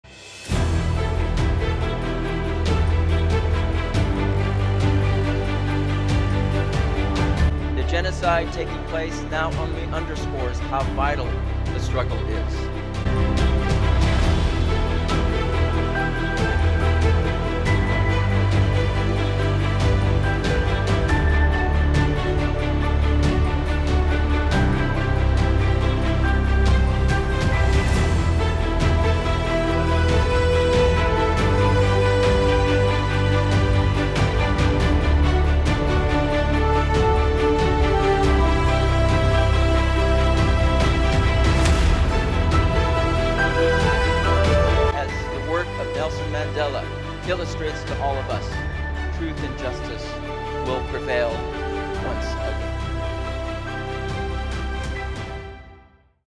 Truth and Justice will Prevail
Tuesday, February 27, 2024, MPAC hosted a ceremony to honor the government of South Africa for their courageous stand at the International Court of Justice (ICJ). This act of courage exemplified their values and commitment to justice in the case against Israel for committing acts of genocide in Gaza. MPAC president, Salam Al-Marayati, presented Ambassador Ndumiso N. Ntshinga with an award for South Africa’s unwavering support of the Palestinian people and their efforts to hold the Israeli government accountable for their egregious, and ongoing atrocities in occupied territories.